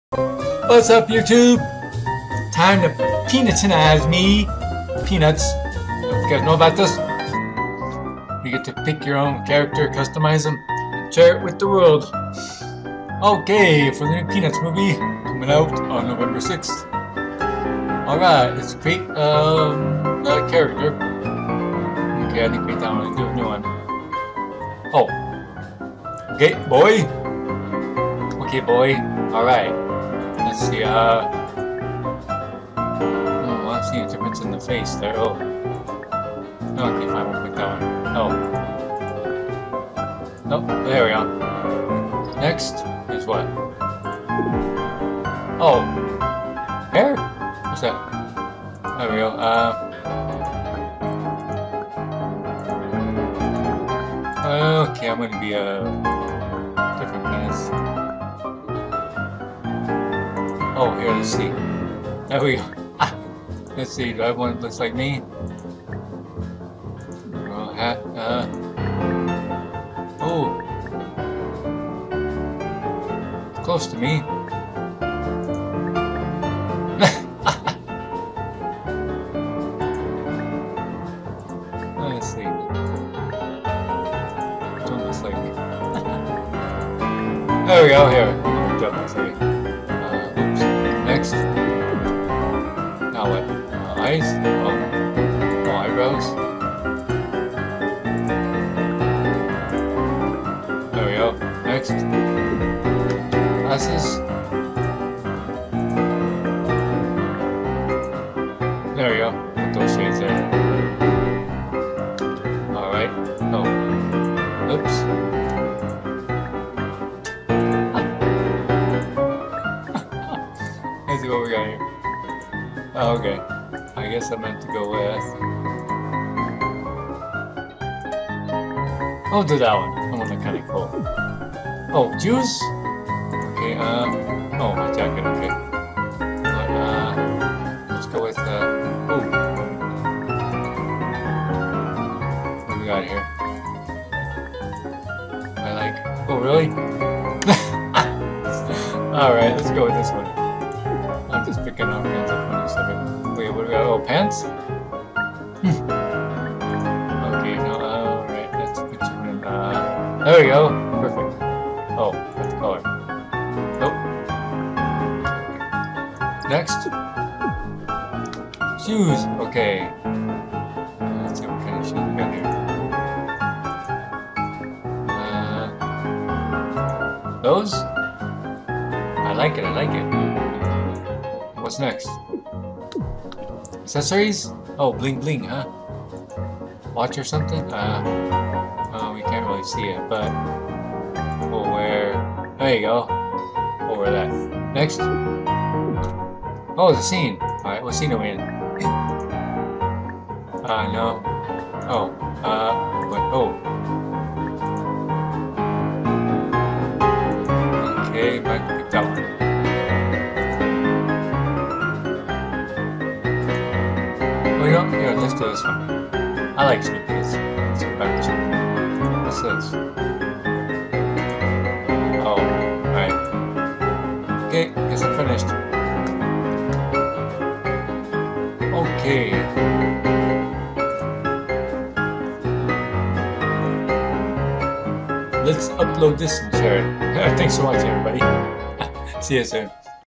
Get Peanutized for new Peanuts movie.
Creating your own Peanuts avatar is easy!  Choose your character's gender, clothes, hair, accessories and more to make your very own Peanutized persona, then share it with the world.